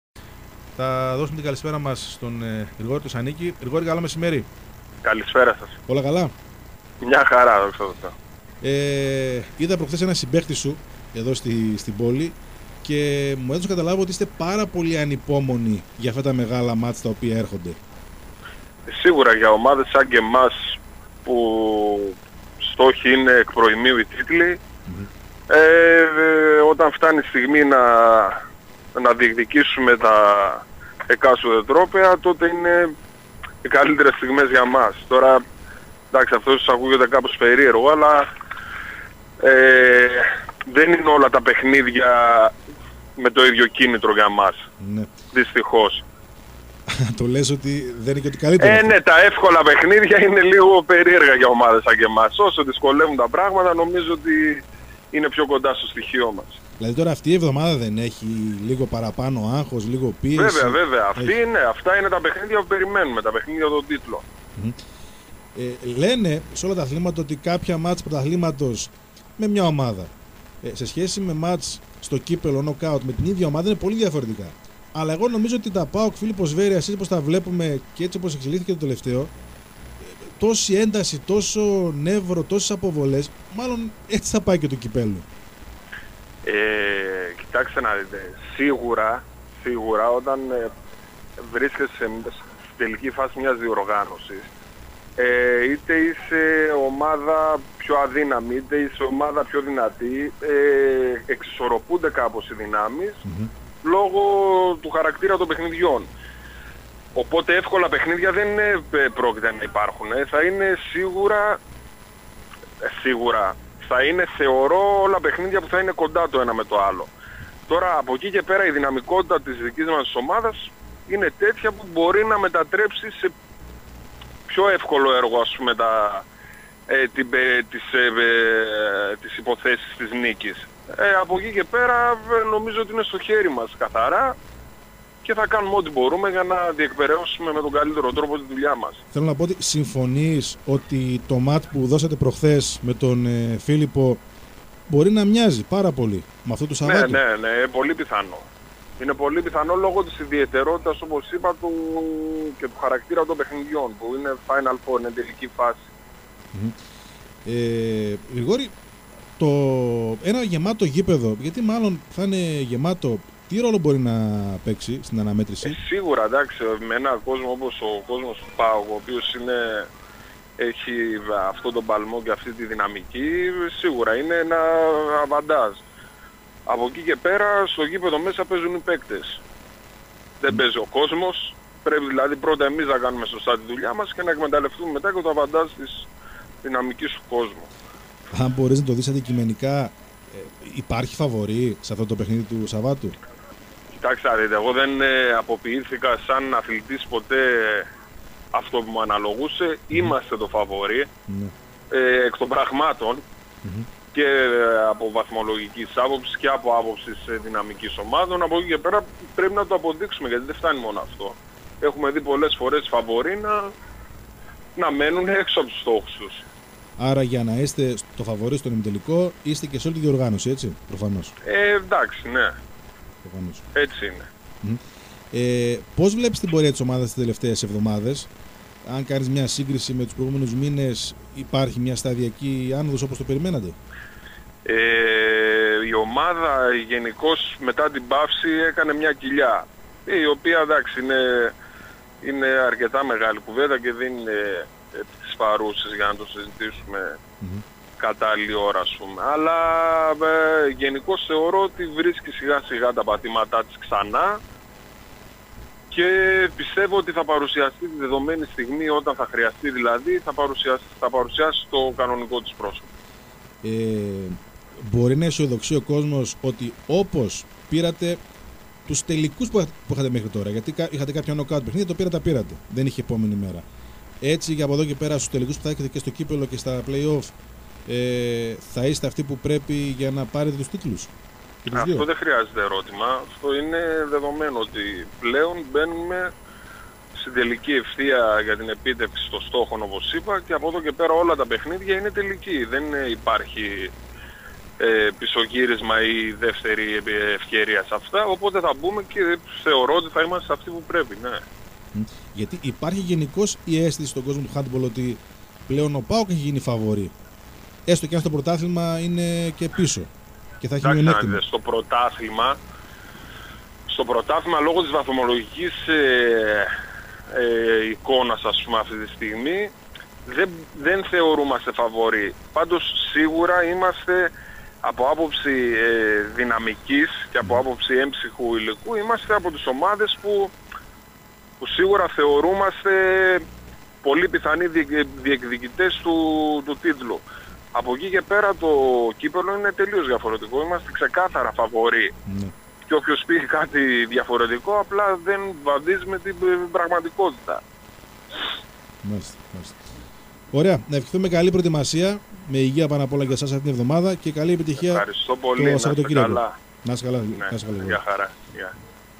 O "Γρηγόρης Σανίκης" στον Libero 107,4
Ο Γρηγόρης Σανίκης φιλοξενήθηκε στον «Libero 107,4»